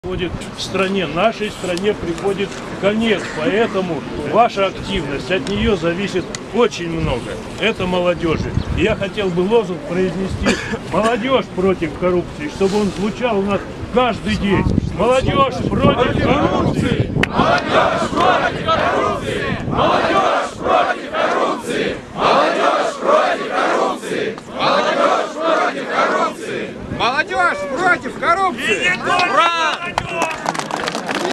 altapress.ru: Митинг против коррупции в Барнауле
26 марта в Барнауле на пл. Свободы прошел митинг против коррупции, организованный представителями оппозиционного политика Алексея Навального. На мероприятии собралось, по разным оценкам, от 300 до 500 человек.